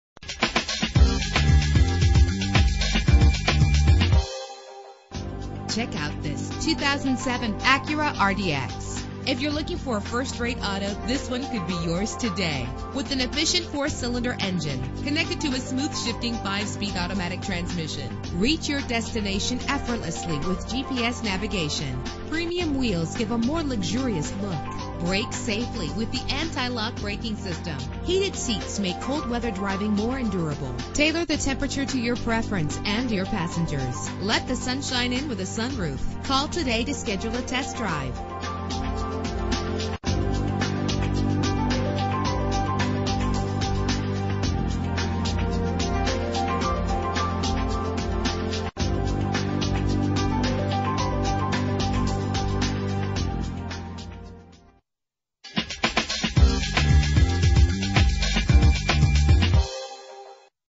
2007 Acura RDX - Lynbrook NY
Year: 2007
Make: Acura
Model: RDX
Trim: Technology
Engine: 2.3 liter inline 4 cylinder 16 valve
Transmission: 5-Speed Automatic
Color: White Diamond Pearl
Mileage: 25119
Address:
6